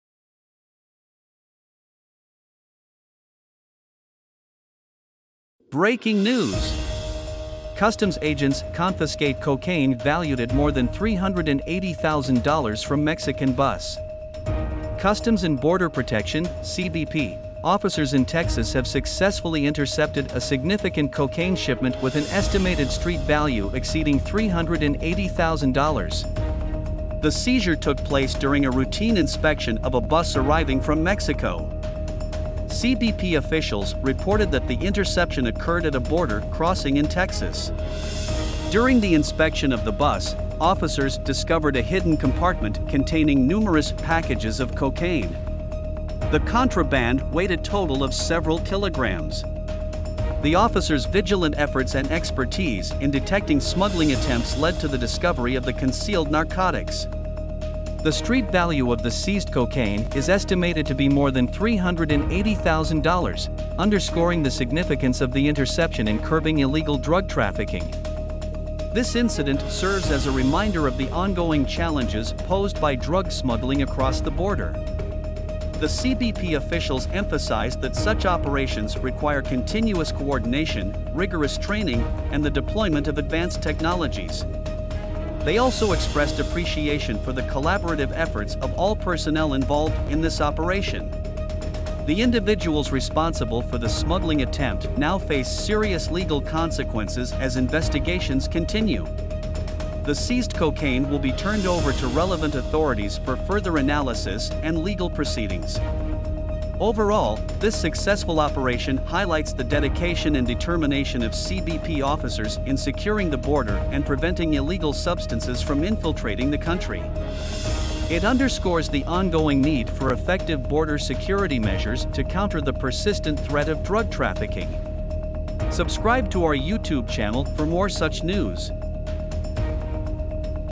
Customs officers seize over $380,000 worth of cocaine off bus from Mexico
Breaking News!

Customs agents confiscate cocaine valued at more than $380,000 from Mexican bus

Customs and Border Protection (CBP) officers in Texas have successfully intercepted a significant cocaine shipment with an estimated street value exceeding $380,000.

The seizure took place during a routine inspection of a bus arriving from Mexico.

CBP officials reported that the interception occurred at a border crossing in Texas.

During the inspection of the bus, officers discovered a hidden compartment containing numerous packages of cocaine.

The contraband weighed a total of several kilograms.

The officers' vigilant efforts and expertise in detecting smuggling attempts led to the discovery of the concealed narcotics.

The street value of the seized cocaine is estimated to be more than $380,000, underscoring the significance of the interception in curbing illegal drug trafficking.

This incident serves as a reminder of the ongoing challenges posed by drug smuggling across the border.

The CBP officials emphasized that such operations require continuous coordination, rigorous training, and the deployment of advanced technologies.

They also expressed appreciation for the collaborative efforts of all personnel involved in this operation.

The individuals responsible for the smuggling attempt now face serious legal consequences as investigations continue.

The seized cocaine will be turned over to relevant authorities for further analysis and legal proceedings.

Overall, this successful operation highlights the dedication and determination of CBP officers in securing the border and preventing illegal substances from infiltrating the country.

It underscores the ongoing need for effective border security measures to counter the persistent threat of drug trafficking.